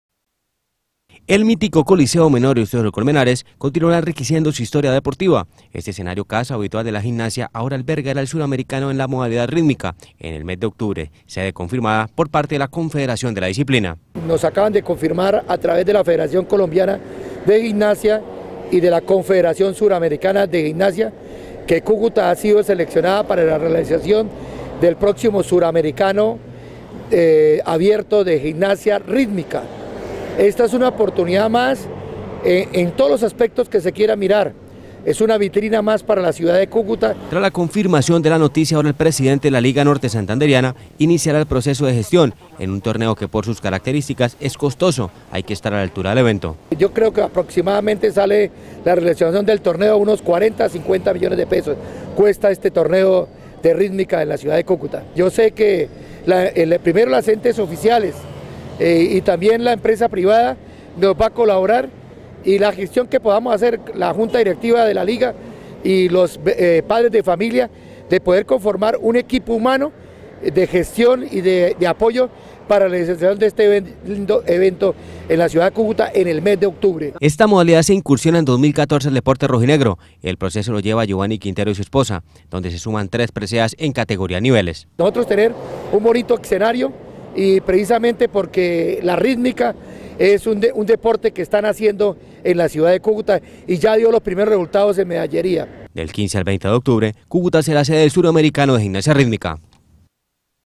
ATN-JULIO 24-CUCUTA SEDE SURAMERICANO DE GIMNASIA ARTISTICA
Del 15 al 20 de octubre, Cucuta albergara el suramericano de Gimnasia Rítmica.